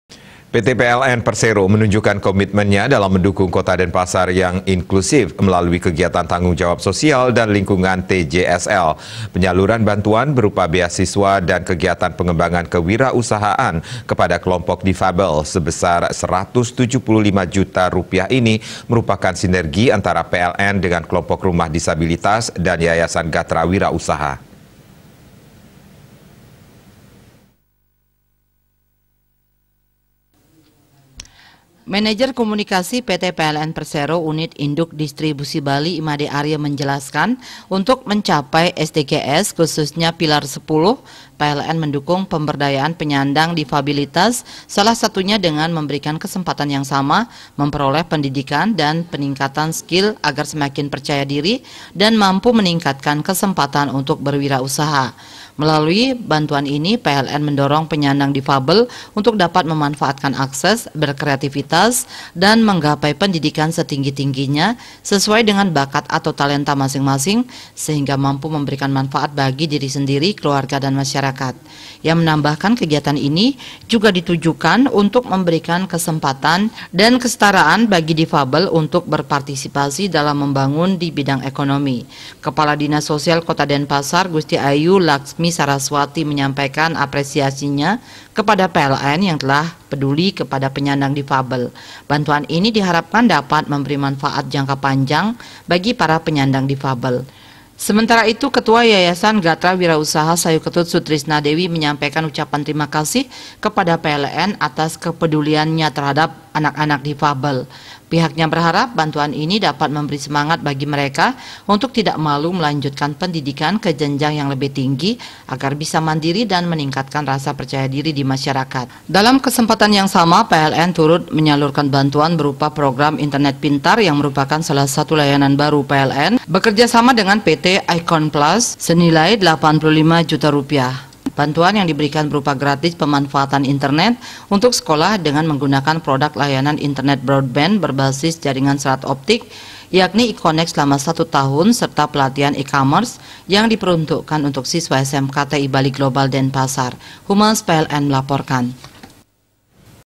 DUKUNG PEMBANGUNAN KOTA INKLUSIF, PLN BERDAYAKAN PENYANDANG DIFABEL DI DENPASAR
P-T P-L-N (PERSERO) MENUNJUKKAN KOMITMENNYA DALAM MENDUKUNG KOTA DENPASAR YANG INKLUSIF, MELALUI KEGIATAN TANGGUNG JAWAB SOSIAL DAN LINGKUNGAN (T-J-S-L). PENYALURAN BANTUAN BERUPA BEASISWA DAN KEGIATAN PENGEMBANGAN KEWIRAUSAHAAN KEPADA KELOMPOK DIFABEL SEBESAR RP 175 JUTA INI MERUPAKAN SINERGI ANTARA P-L-N DENGAN KELOMPOK RUMAH DISABILITAS DAN YAYASAN GATRA WIRAUSAHA.